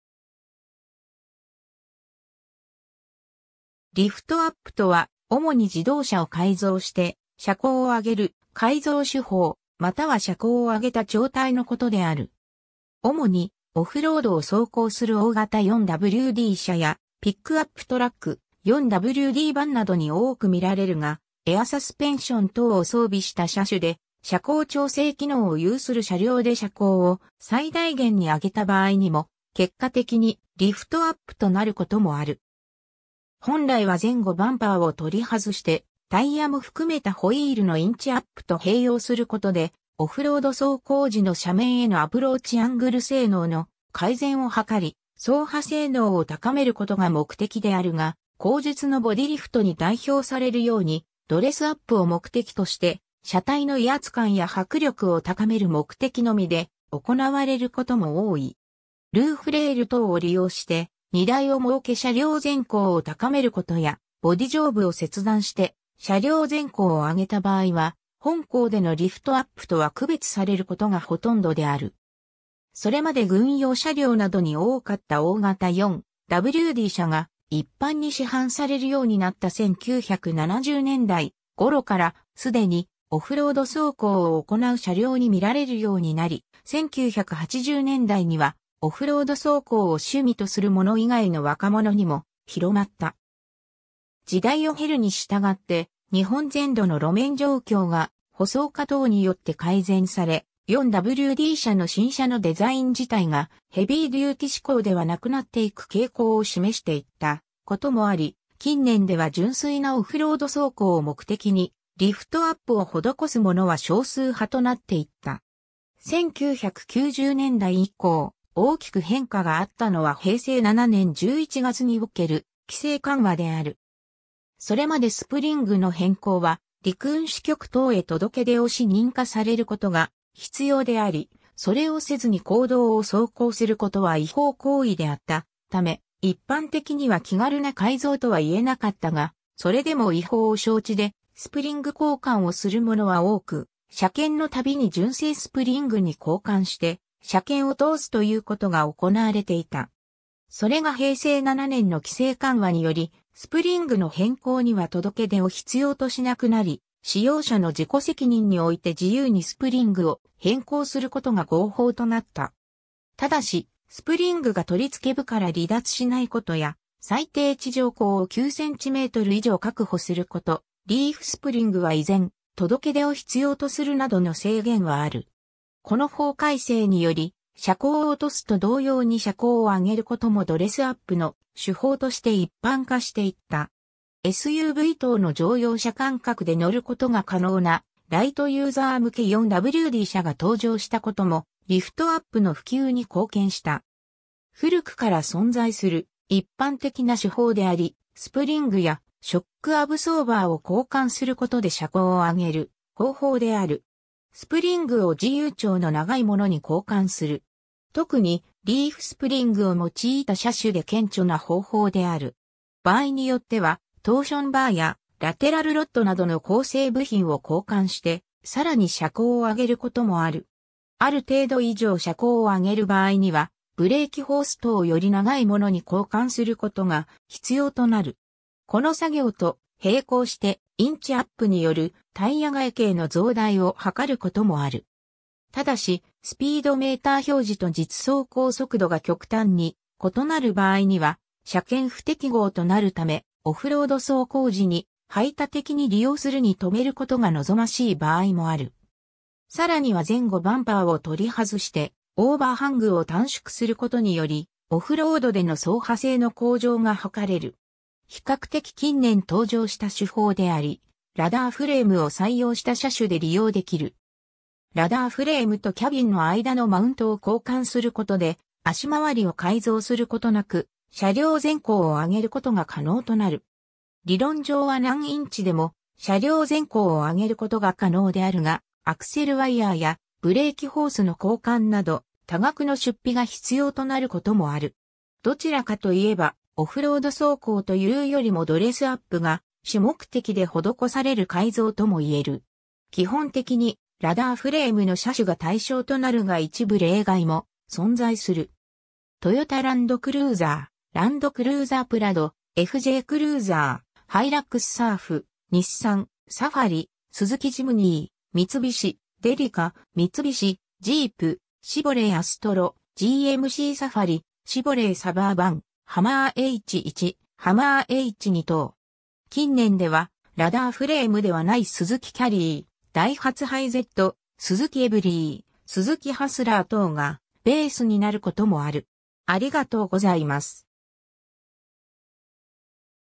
リフトアップ
リフトアップ（ハイリフトとも言う）とは、主に自動車を改造して車高（最低地上高）を上げる改造手法、または車高を上げた状態のことである。
 主にオフロードを走行する大型4WD車やピックアップトラック、4WDバンなどに多く見られるが、エアサスペンション等を装備した車種で車高調整機能（ハイトコントロール）を有する車両で車高を最大限に上げた場合にも結果的にリフトアップとなることもある。
 本来は前後バンパーを取り外してタイヤも含めたホイールのインチアップと併用する事で、オフロード走行時の斜面へのアプローチアングル性能の改善を図り、走破性能を高める事が目的であるが、後述のボディリフトに代表されるようにドレスアップを目的として車体の威圧感や迫力を高める目的のみで行われる事も多い。
 ルーフレール等を利用して荷台を設け車両全高を高める事や、ボディ上部を切断（多くは板金などの処理をして再び接合）して車両全高を上げた場合は、本稿でのリフトアップとは区別されることがほとんどである。
 それまで軍用車両などに多かった大型4WD車が一般に市販されるようになった1970年代ごろから既にオフロード走行を行う車両に見られるようになり、1980年代にはオフロード走行を趣味とする者以外の若者にも広まった。
時代を経るに従って、日本全土の路面状況が舗装化等によって改善され、4WD車の新車のデザイン自体がヘビーデューティ指向では無くなっていく傾向（独立懸架やモノコック構造の採用など）を示していった事もあり、近年では純粋なオフロード走行を目的にリフトアップを施す者は少数派となっていった。
 1990年代以降、大きく変化があったのは平成7年（1995年）11月における規制緩和である。
それまでスプリングの変更は陸運支局等へ届出をし認可される（いわゆる「公認」を取得する）ことが必要であり、それをせずに公道を走行することは違法行為であったため、一般的には気軽な改造（もしくはドレスアップ）とは言えなかったが、それでも違法を承知でスプリング交換をするものは多く、車検のたびに純正スプリングに交換して車検を通すということが行われていた。
それが平成7年の規制緩和により、スプリングの変更には届出を必要としなくなり、使用者の自己責任において自由にスプリング（コイル）を変更することが合法となった。
ただし、スプリングが取り付け部から離脱しないこと（遊ばないこと）や、最低地上高を9cm以上確保すること、リーフスプリング(板バネ)は依然、届出を必要とするなどの制限はある。
この法改正により、車高を落とすと同様に車高を上げることもドレスアップの手法として一般化していった。
SUV等の乗用車感覚で乗る事が可能なライトユーザー向け4WD車が登場した事もリフトアップの普及に貢献した。
 古くから存在する一般的な手法であり、スプリングやショックアブソーバーを交換する事で車高を上げる方法である。
 スプリングを自由長の長い物に交換する。
特にリーフスプリングを用いた車種で顕著な方法である。
場合によってはトーションバーやラテラルロッドなどの構成部品を交換して更に車高を上げることもある。
 ある程度以上車高を上げる場合にはブレーキホース等をより長い物に交換する事が必要となる。
 この作業と並行してインチアップによるタイヤ外径の増大を図る事もある。
但し、スピードメーター表示と実走行速度が極端に異なる場合には車検不適合となる為、オフロード走行時に排他的に利用するに留める事が望ましい場合もある。
 更には前後バンパーを取り外してオーバーハングを短縮する事により、オフロードでの走破性の向上が図れる。
 比較的近年登場した手法であり、ラダーフレームを採用した車種で利用出来る。
ラダーフレームとキャビンの間のマウントを交換する事で、足回りを改造することなく車両全高を上げる事が可能となる。
理論上は何インチでも車両全高を上げる事が可能であるが、アクセルワイヤーやブレーキホースの交換など多額の出費が必要となる事もある。
 どちらかと言えば、オフロード走行と言うよりもドレスアップが主目的で施される改造とも言える。
 基本的にラダーフレームの車種が対象となるが一部例外も存在する。
トヨタ・ランドクルーザー、ランドクルーザープラド、FJクルーザー、ハイラックスサーフ、日産・サファリ、スズキ・ジムニー、三菱・デリカ、三菱・ジープ、シボレー・アストロ、GMC・サファリ、シボレー・サバーバン、ハマー・H1、ハマー・H2等。
近年で...